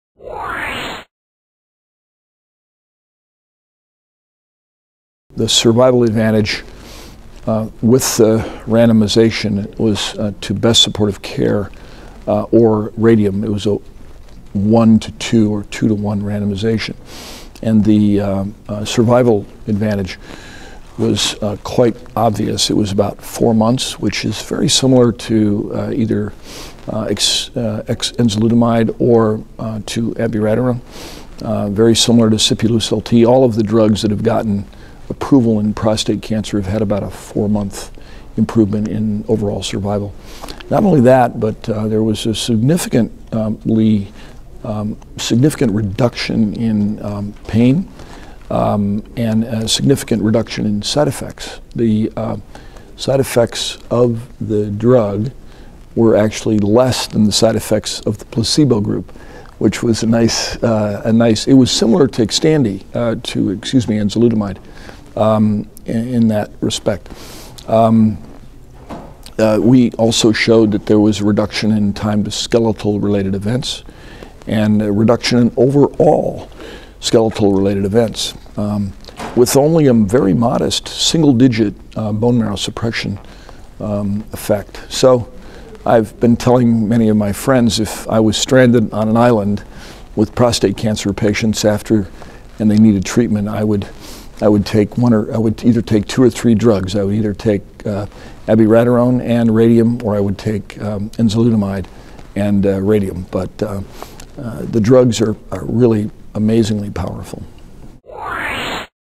Dr. Nicholas Vogelzang Discusses Radium-223 in Prostate Cancer
Nicholas J. Vogelzang, MD, member of the US Oncology Network, site research leader for Comprehensive Cancer Centers of Nevada, professor of medicine at the University of Nevada School of Medicine, discusses an updated analysis of the phase III ALSYMPCA trial that examined radium-223 dichloride in patients with castration-resistant prostate cancer (CRPC).